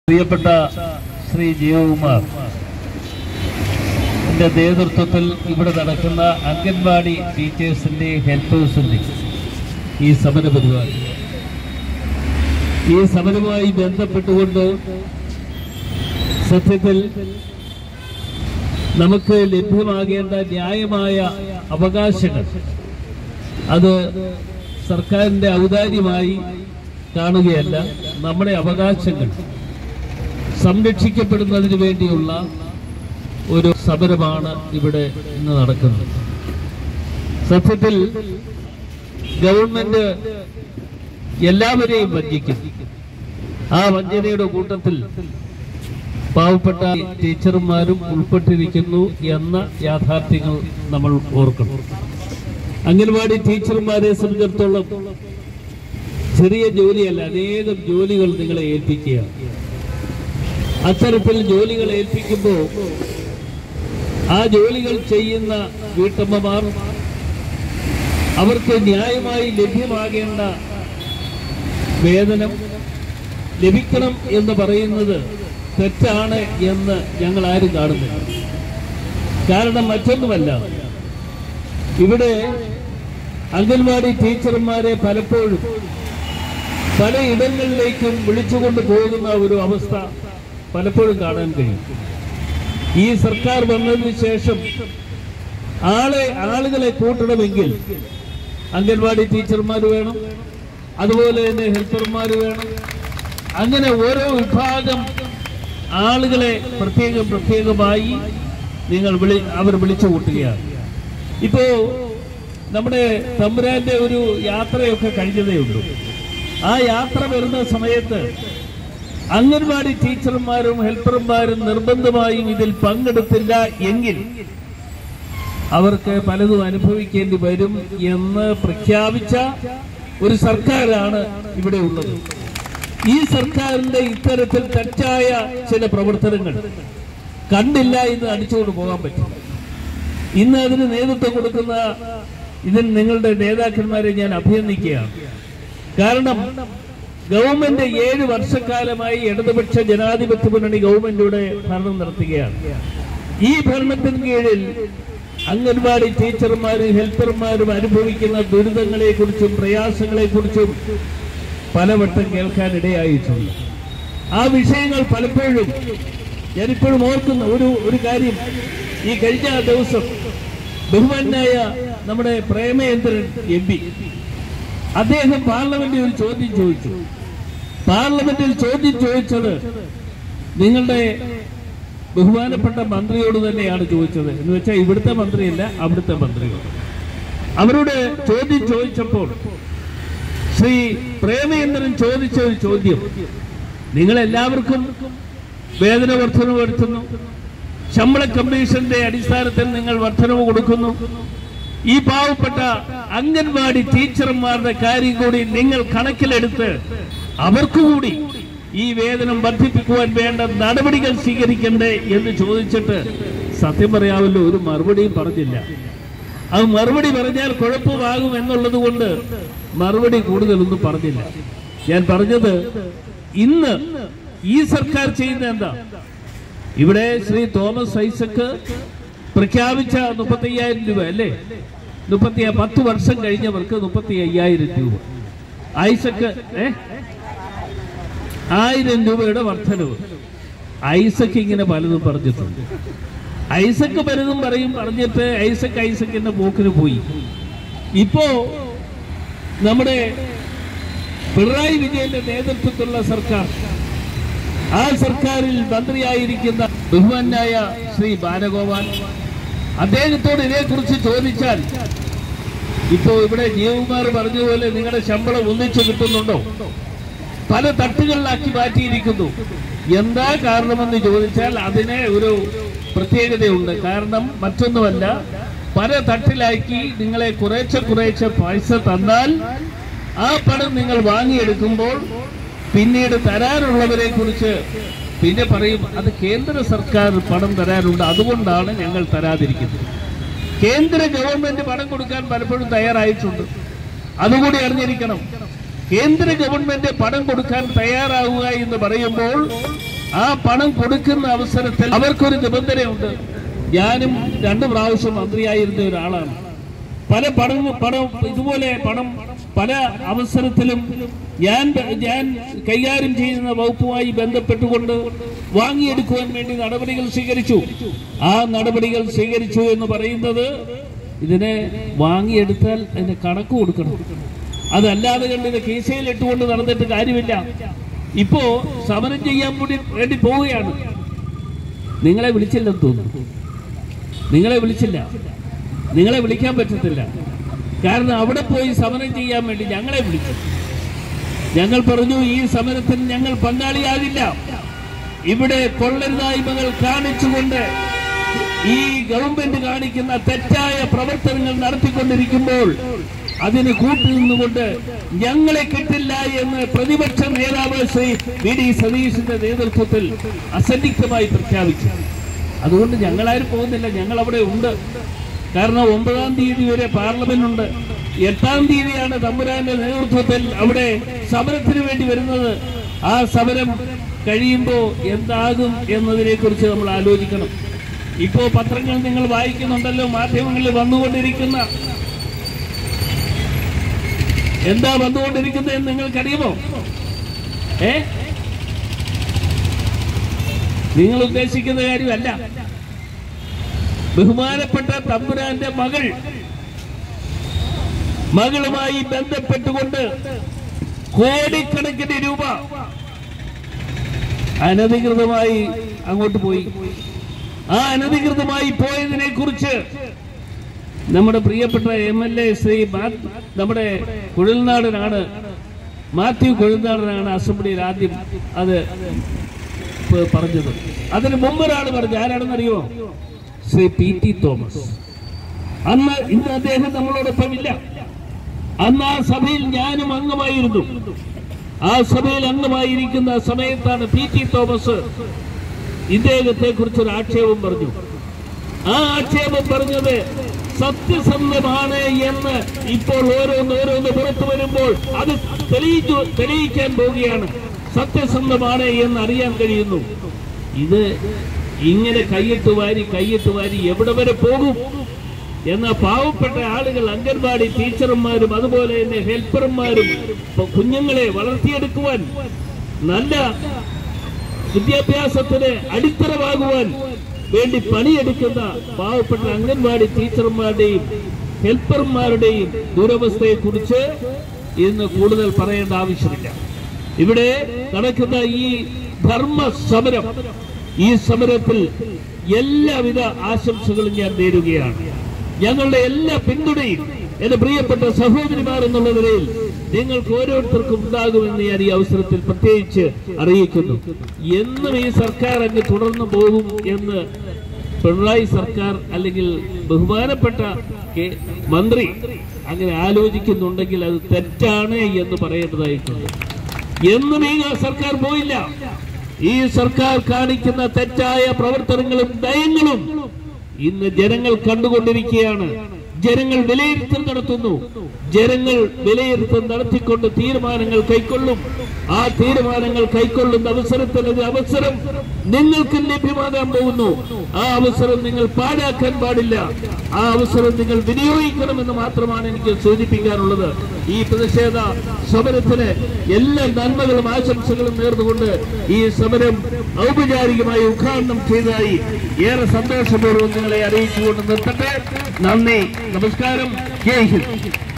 അടൂർ പ്രകാശ് MP സെക്രട്ടറിയേറ്റ് സമരം
അംഗൻവാടി ജീവനക്കാരുടെ സമരത്തിന് Adoor Prakash MP എത്തിയപ്പോൾ.